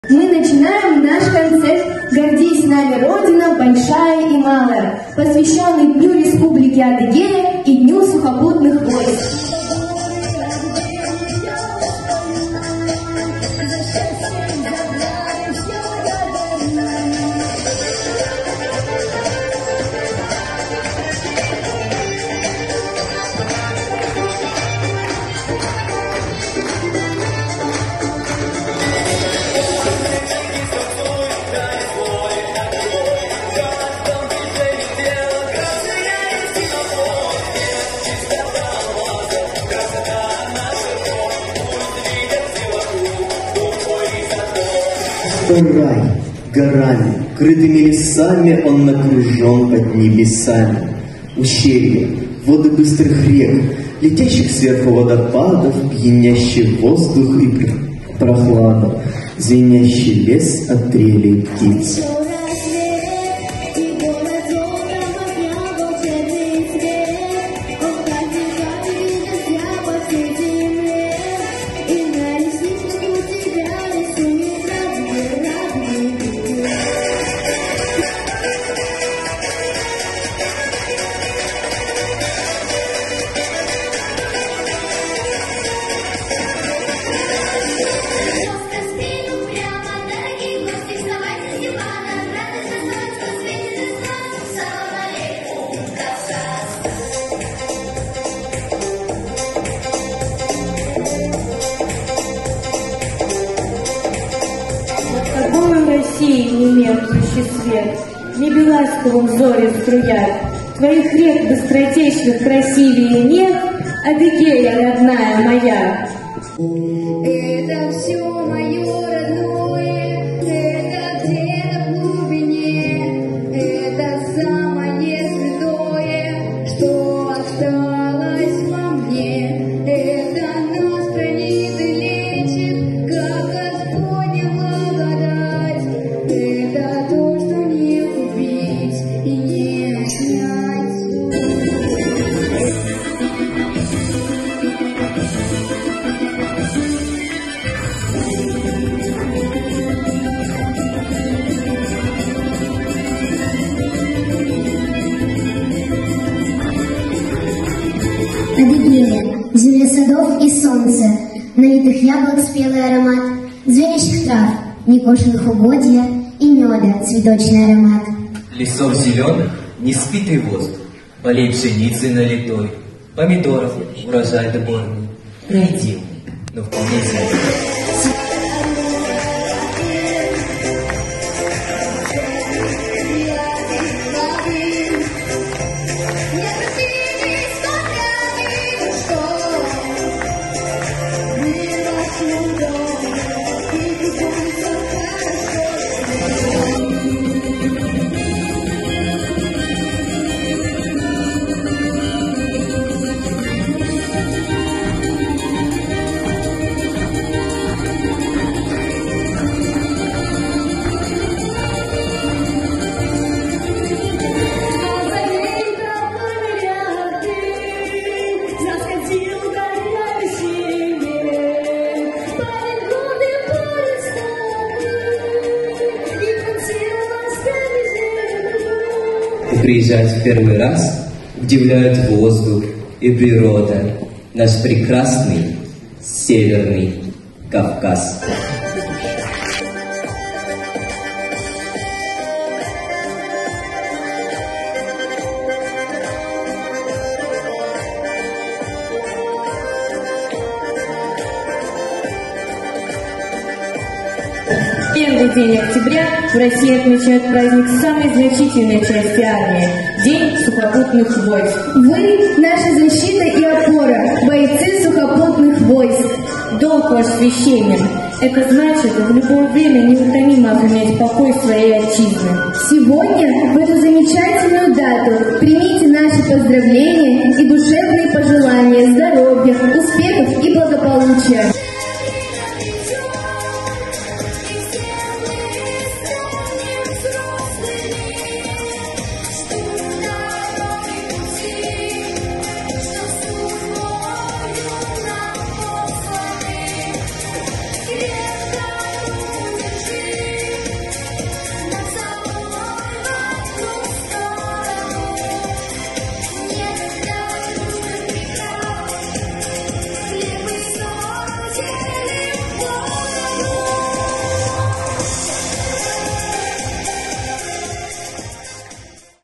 Праздничный концерт «Гордись нами, Родина, большая и малая!
Для военнослужащих в/ч 13714 прошел праздничный концерт «Гордись нами, Родина, большая и малая!

Концерт посвящен Дню сухопутных войск и Дню Республики Адыгея!

Подготовили и провели концерт преподаватели и учащиеся Детской школы искусств №1 г. Майкопа.